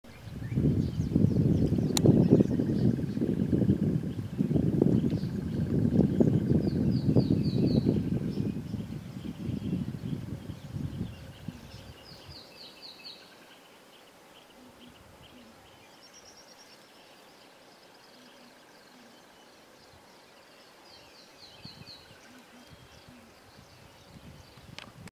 Southern Ground Hornbill; S.Africa. Кафрский рогатый ворон; ЮАР (2203sp)
(Bucorvus leadbeateri); South Africa; Kruger National Park; December 21, 2010. ЮАР, Нац. парк Крюгер; 21 декабря 2010.